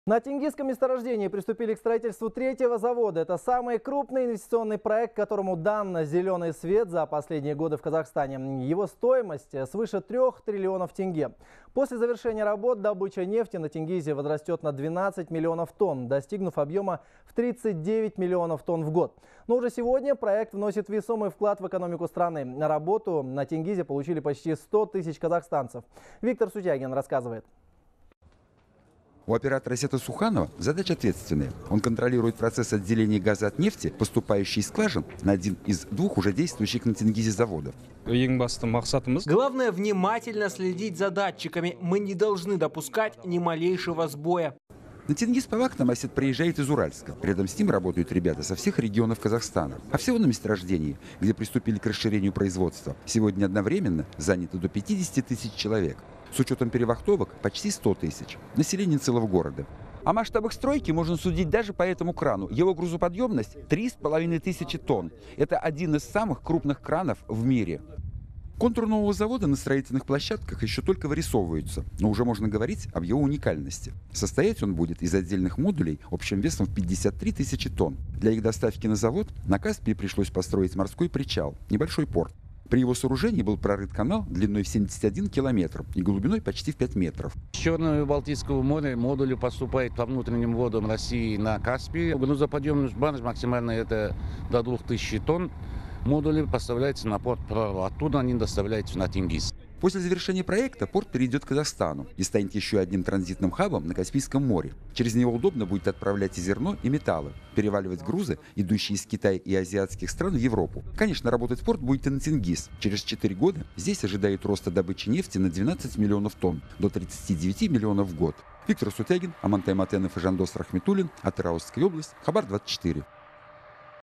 Завод стоимостью свыше 3 триллионов тенге строят на Тенгизе
На Тенгизском месторождении приступили к строительству третьего завода. Это самый крупный инвестиционный проект, которому дан «зеленый свет» за последние годы в Казахстане. Его стоимость – свыше трех триллионов тенге.

После завершения работ добыча нефти на Тенгизе возрастет на 12 миллионов тонн, достигнув объема в 39 миллионов тонн в год. Но уже сегодня проект вносит весомый вклад в экономику страны – работу на Тенгизе получили почти сто тысяч казахстанцев.

У оператора Асета Суханова задача ответственная – он контролирует процесс отделения газа от нефти, поступающей из скважин на один из двух уже действующих на Тенгизе заводов.